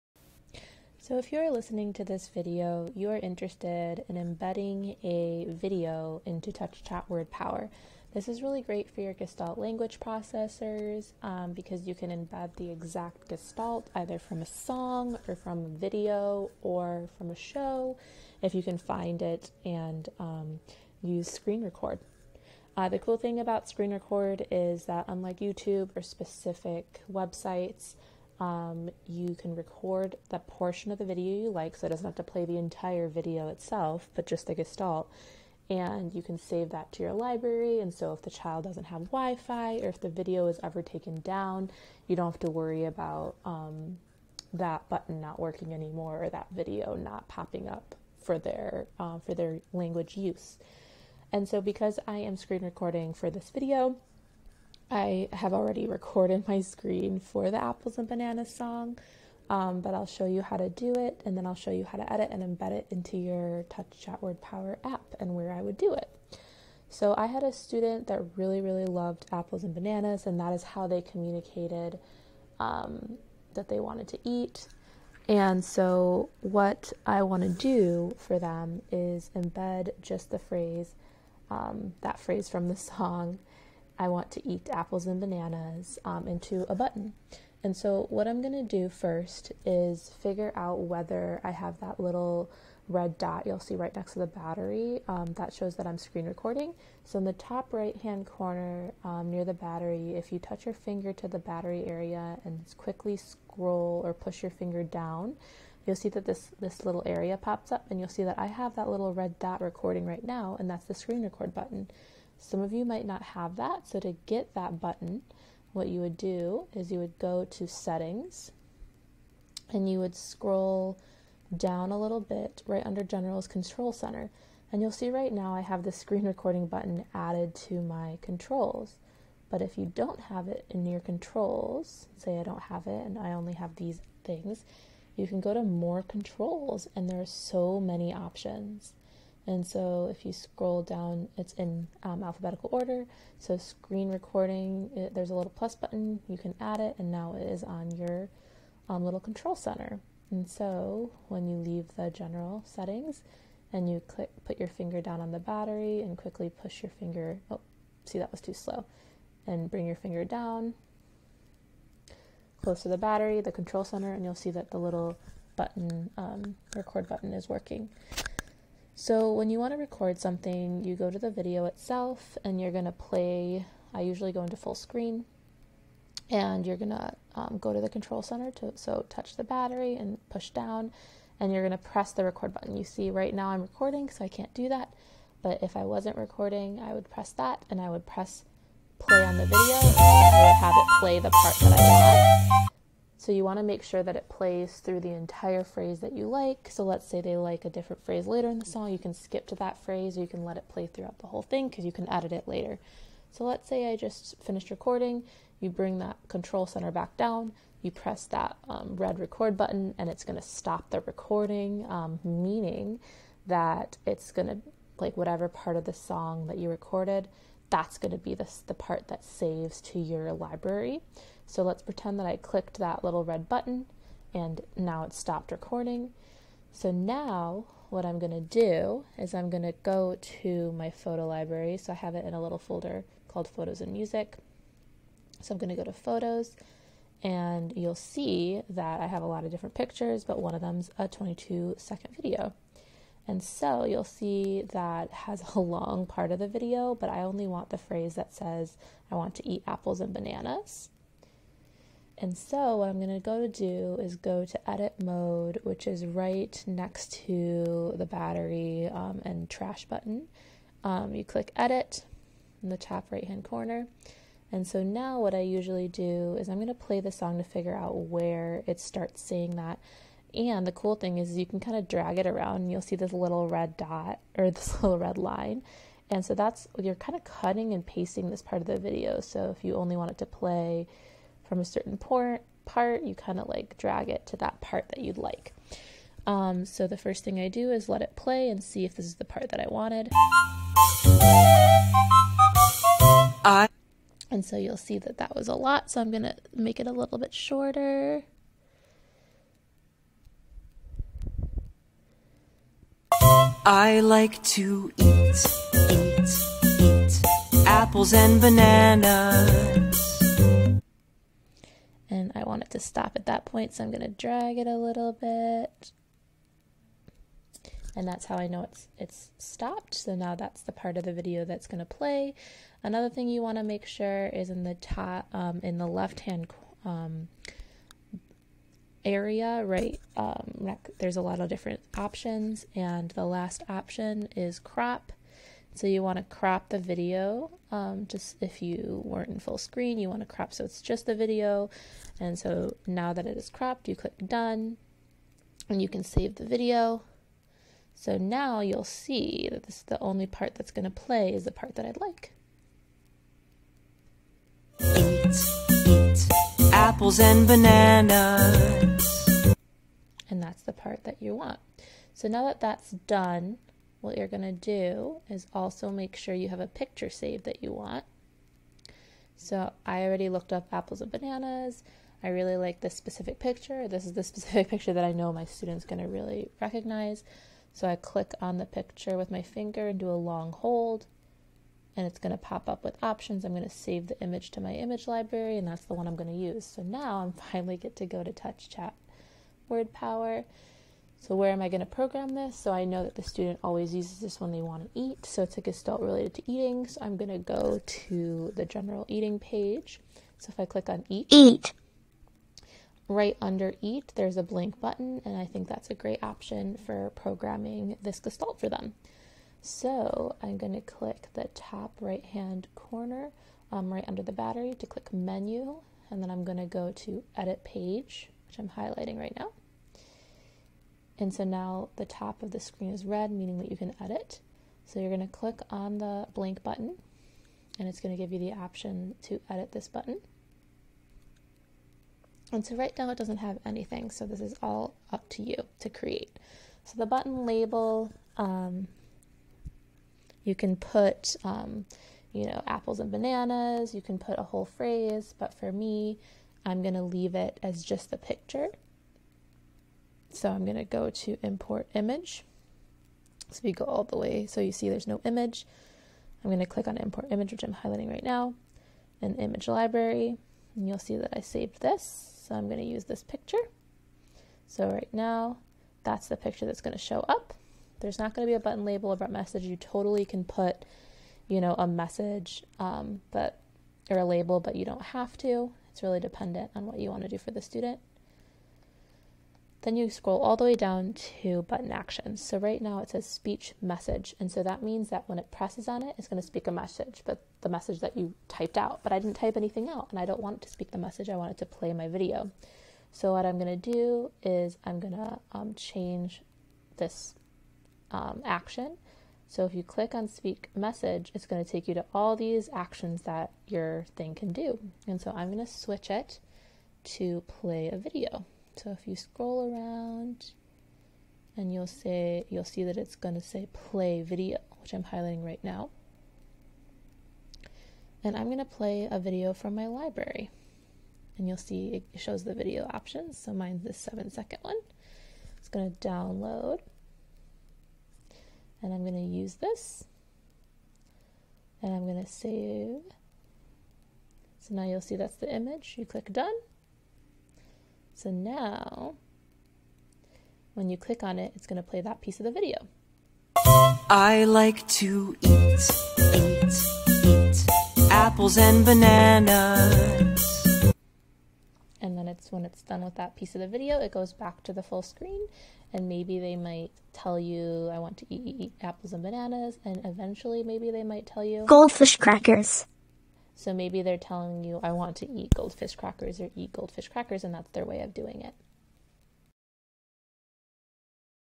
TouchChat WordPower: Embedding a video into a button using screen recording (Tips for GLP)
This video helps you learn how to embed a video into a button on TCWP. This is great for Gestalt Language Processors.